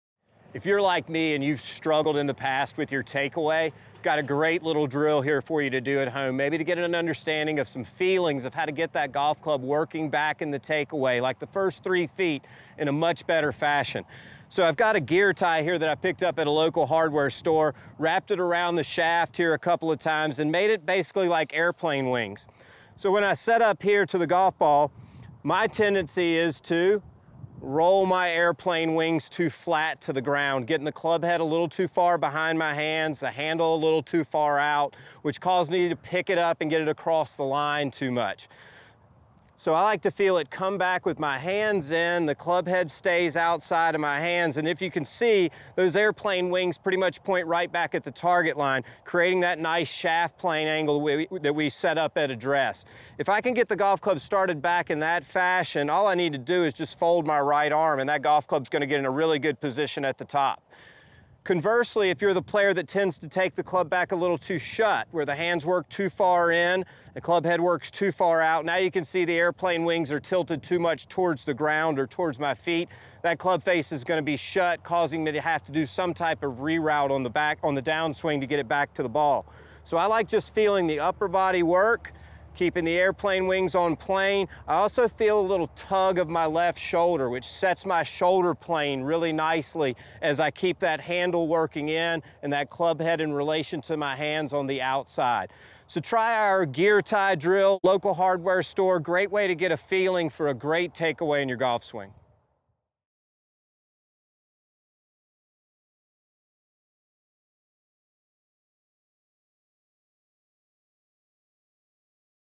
Unlock the secret to a flawless golf swing with TeamTitleist!
Unlock the secret to a flawless golf swing with TeamTitleist! Discover a simple yet powerful drill designed to help you perfect your takeaway. Learn how the first few feet of your swing can set you up for success and improve your overall performance. Watch now and start mastering your golf swing today!